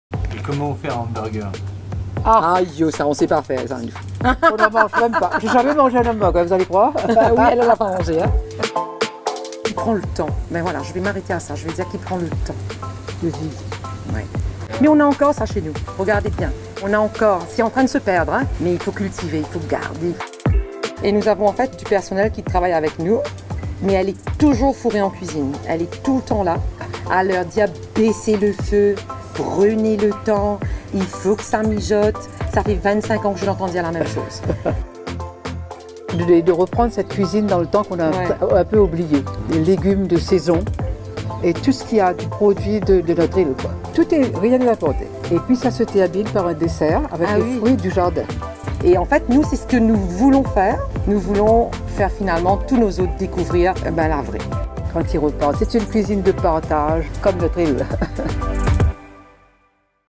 Discover Real Creole Cuisine
These dishes are supposed to be shared. Just like our island!

Escale Créole is more than a local restaurant. It’s your chance to discover the heart and soul of Mauritius. Meet our people, taste locally-grown ingredients and learn about the joys of slow living.

Mum and daughter duo, Marie-Christine and Majo, are here to tell you more.